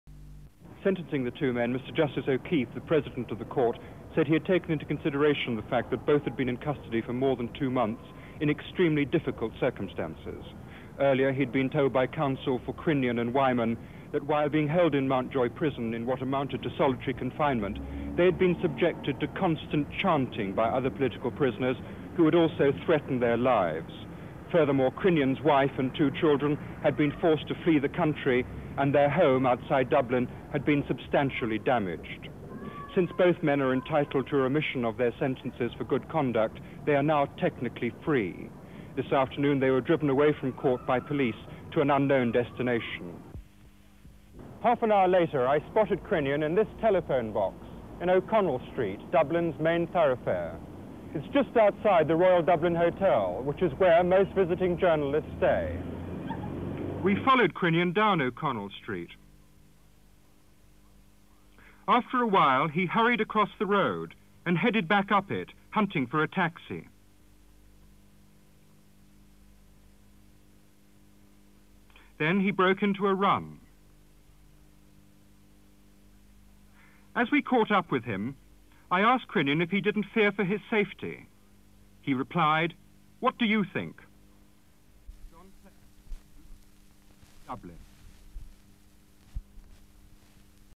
SYND 27-2-73 DUBLIN TRAIL CONVICTIONS OF ENGLISH SPY PATRICK CRINNION
(27 Feb 1973) Dublin TrIal convictions of English spy Patrick Crinnion